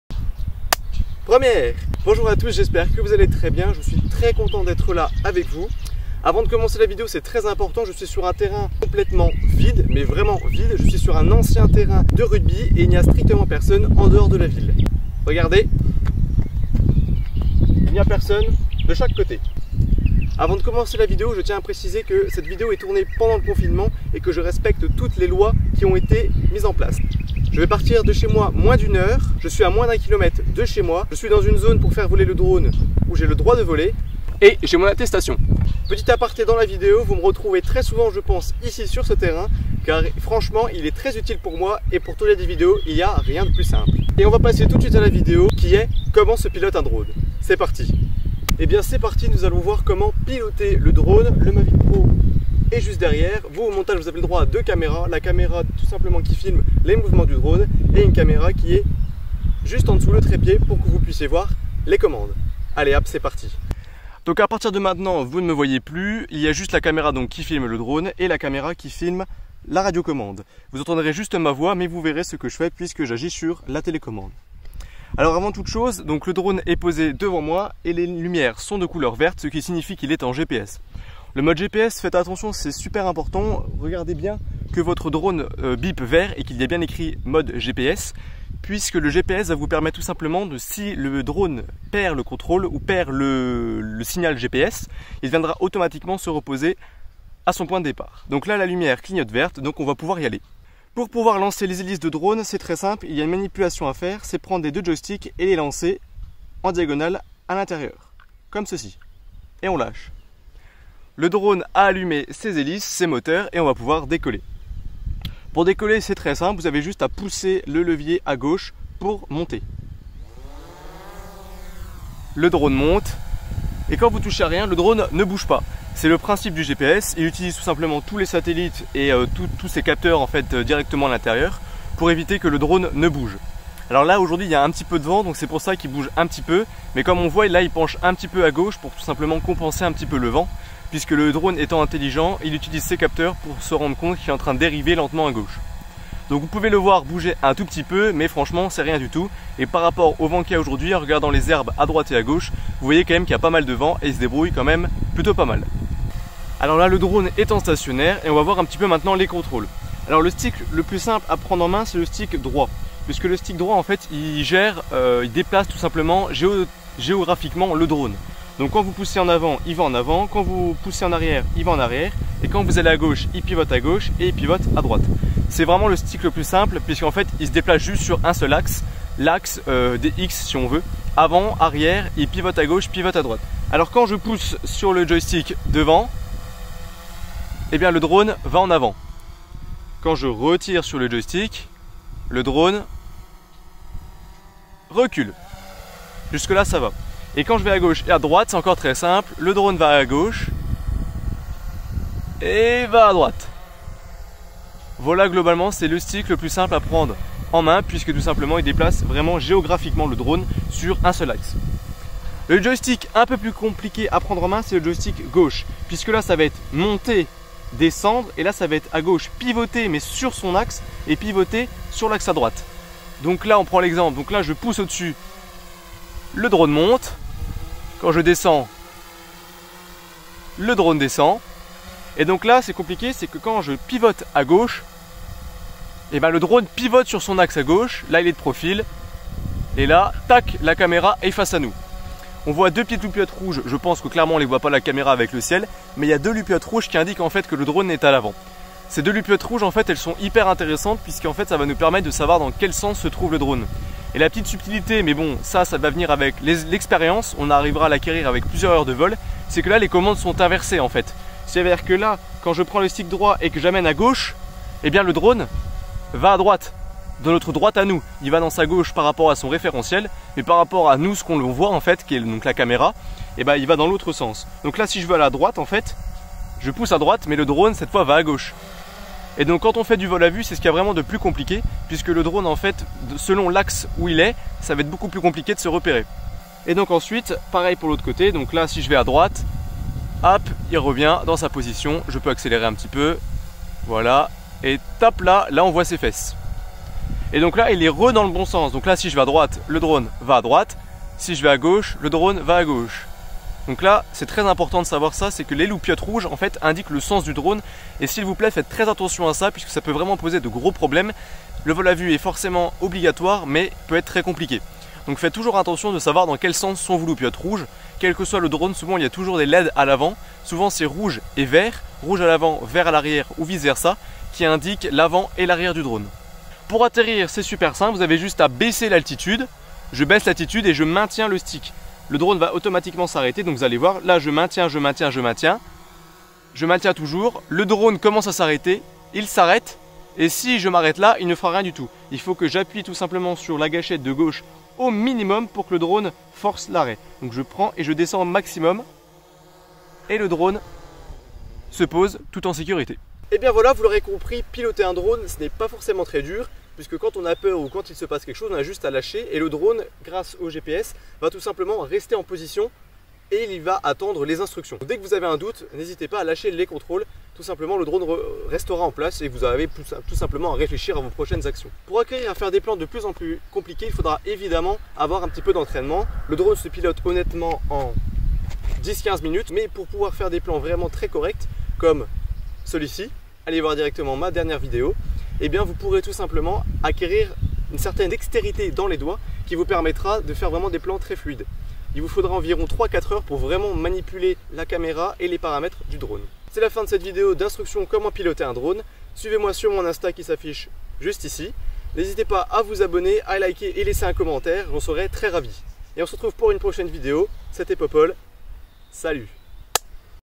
COMMENT PILOTER UN DRONE ?!
Bonjour à tous, aujourd'hui je vous explique comment piloter un drone.
Piloter un drone n'est pas forcément très compliqué, mais il vous faudra de l'entrainement pour acquérir une maitrise totale de l'appareil, et pour la réalisation de plan cinématographique.